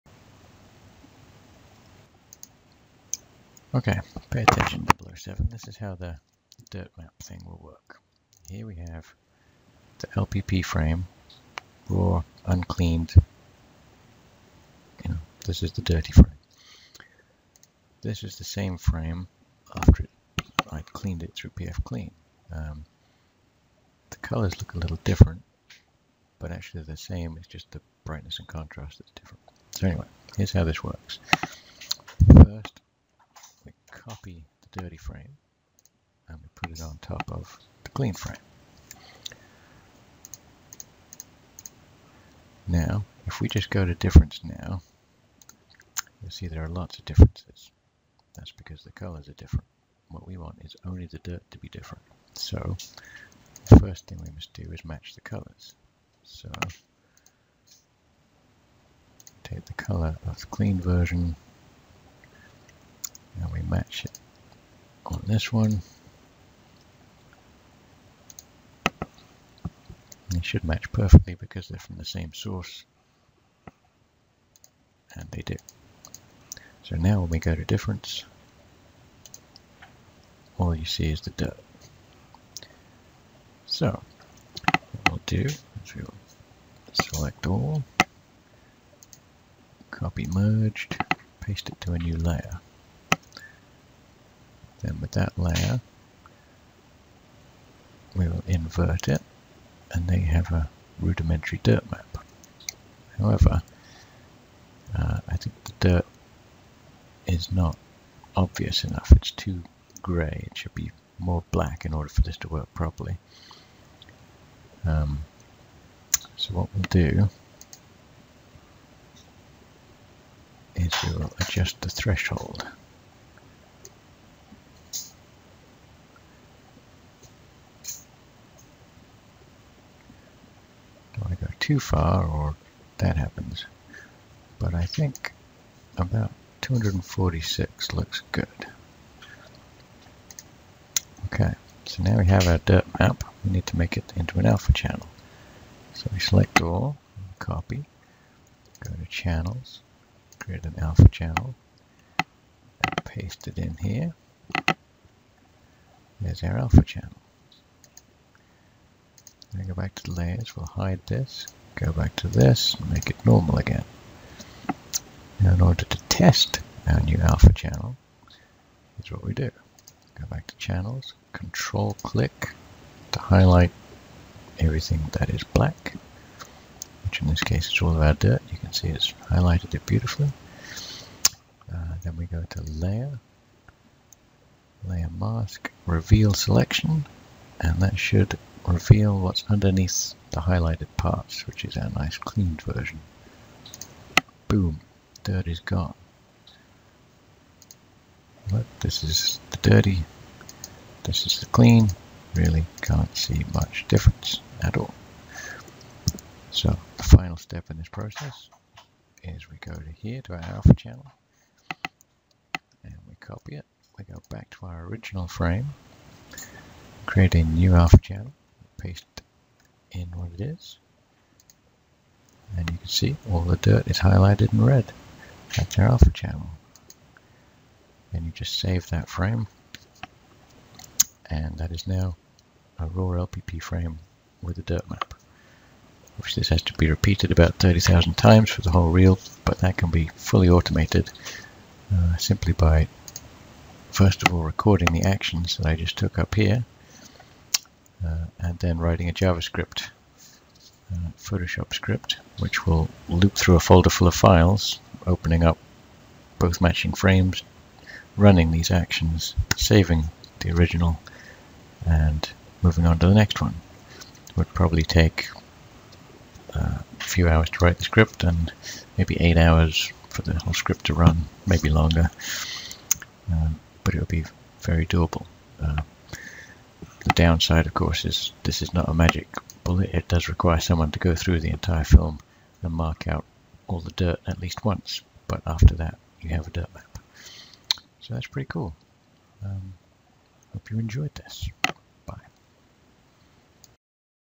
Create your own Dirt Map
If you can perfectly register two identical frames, AND perfectly match their colors and one of them is clean, but the other is dirty, you can generate a dirt map for it in Photoshop. I apologize for the terrible audio quality...